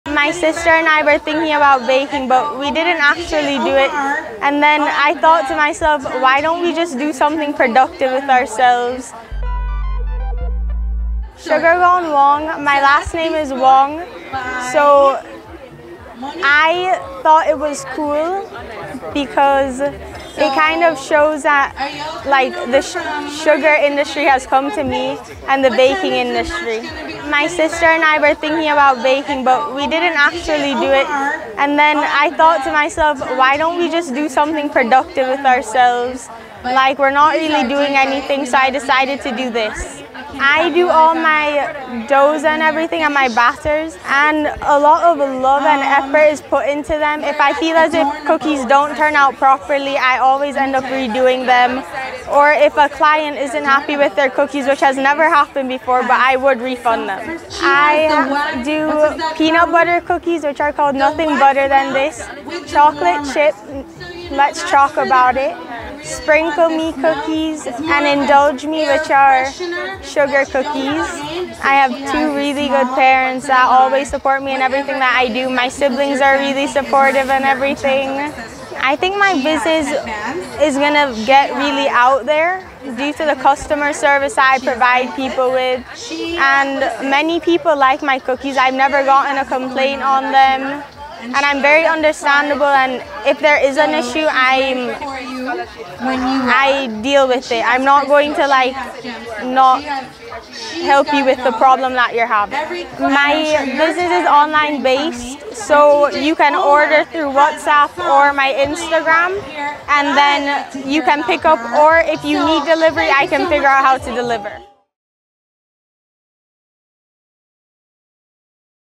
Sugar Gone Wong – Let’s ‘Choc-O-Bout-It’
Fourteen-year-old Sydney Wong is not only baking up sweet treats, but she is also displaying her literary prowess through her cookie business. Wong is the owner of Sugar Gone Wong – a Kingston-based cookie business. She was among some 35 entrepreneurs at the Jamaica Art and Gift Festival (JAGFest) hosted on Murray’s Farm in Toll Gate, Clarendon, on Sunday…